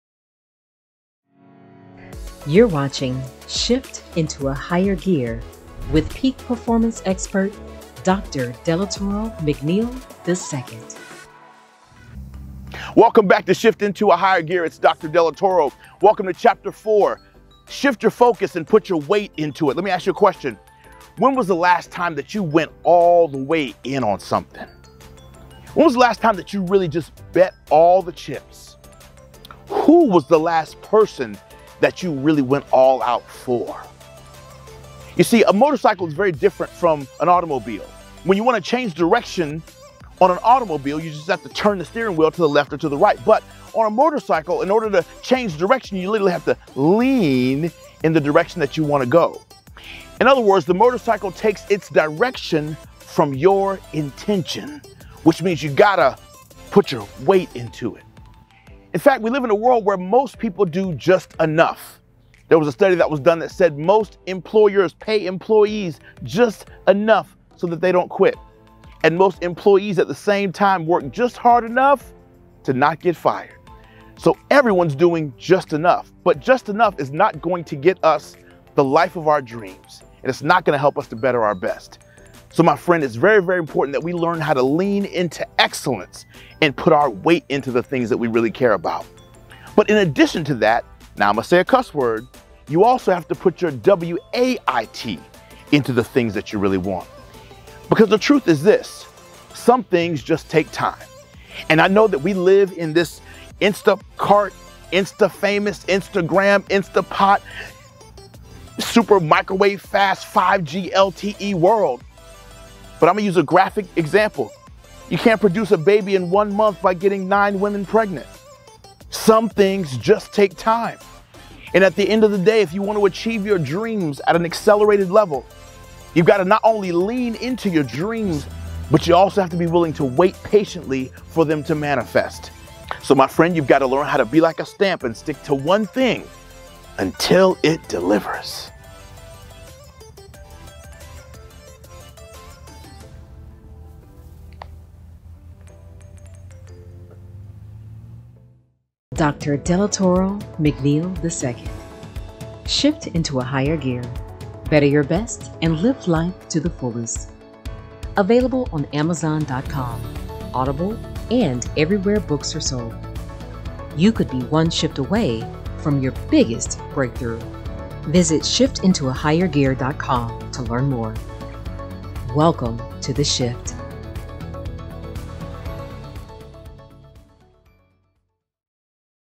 Chapter 4: Shift Your Focus and Put Your Weight Into It
FOCUS! Even reading that word alone, some of you may be tempted to jump to something else. That can be a major issue in life. We have to master the art of focusing and sticking to one thing until it delivers. Allow me to help you discover the root of your intentions, as well as how you can put your weight into the things you desire. Living your dreams is possible; you’ve just got to give up the act of giving up! (See what I did there?) Let’s get intentional and regain our focus!